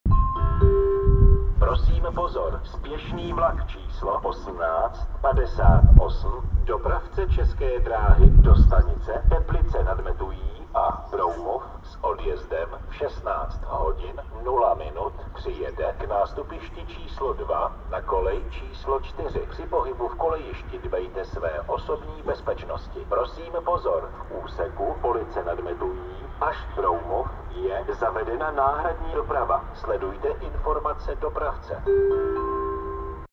HaVIS Náchod, Sp 1858 do stanice Broumov přijede. Police n.Met. - Broumov ND - hlášení dle nové sm.
Informační systém HaVIS hlásí v Náchodě informaci, že Sp 1858 do stanice Broumov přijede. Police nad Metují - Broumov zavedena ND. - Hlášení dle nové směrnice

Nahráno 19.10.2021